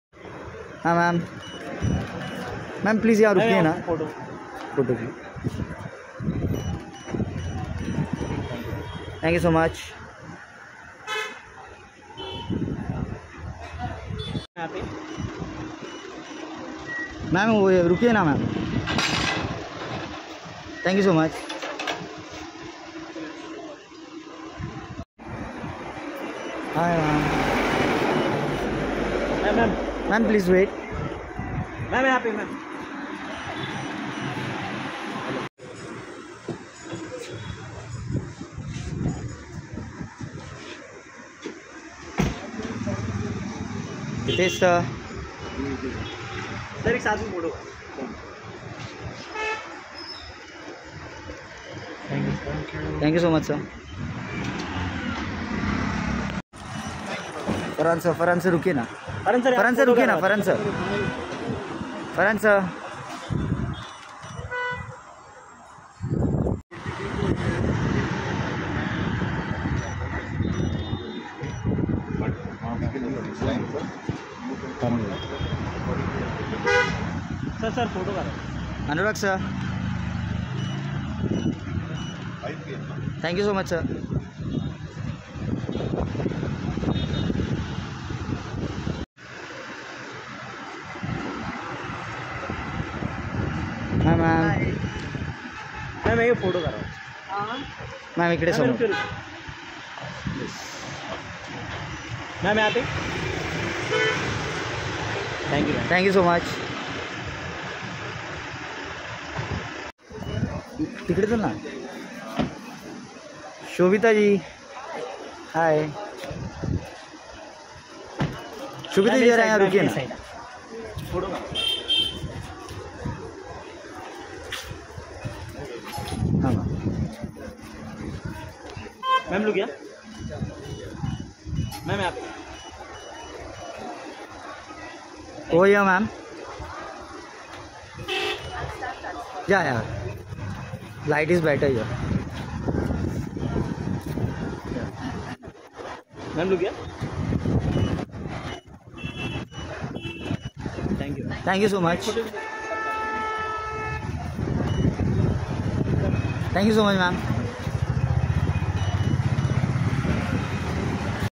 Made In Heaven 2 Team At Zoya Akhtar House Tonight- Shobhita,Mona, Farahan With Wife, Anurag Kashayp
Made In Heaven 2 Team Party At Zoya Akhtar House Tonight - Shobhita Dhulipala,Mona Singh, Farahan Akther With Wife, Anurag Kashayp Many More At Screening